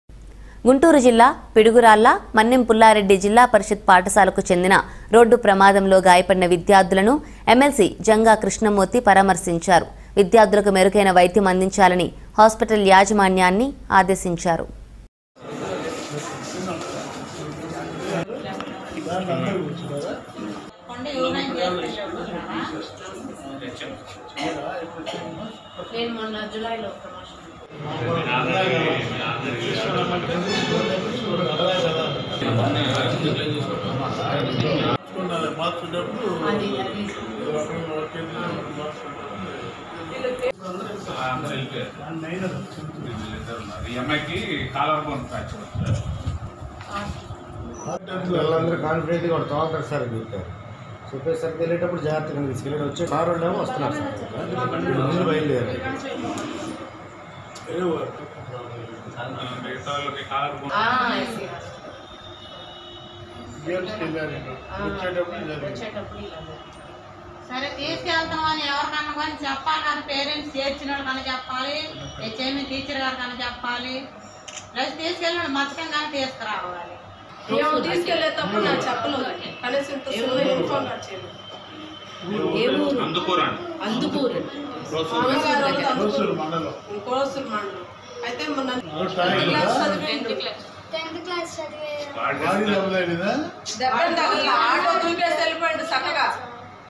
పిడుగురాళ్ళ రోడ్డు ప్ర‌మాదంలో గాయ‌ప‌డిన విద్యార్థుల‌ను ఎమ్మెల్సీ జంగా కృష్ణమూర్తి ప‌రామ‌ర్శించారు
.గుంటూరు జిల్లా పిడుగురాళ్ళ  మన్నెం పుల్లారెడ్డి జిల్లా పరిషత్ పాఠశాలకు చెందిన రోడ్డు ప్ర‌మాదంలో గాయ‌ప‌డిన విద్యార్థుల‌ను    ఎమ్మెల్సీ జంగా కృష్ణమూర్తి ప‌రామ‌ర్శించారు.  విద్యార్థులకు మెరుగైన వైద్యం అందించాలని హాస్పటల్ యాజమాన్యాన్ని ఆదేశించారు.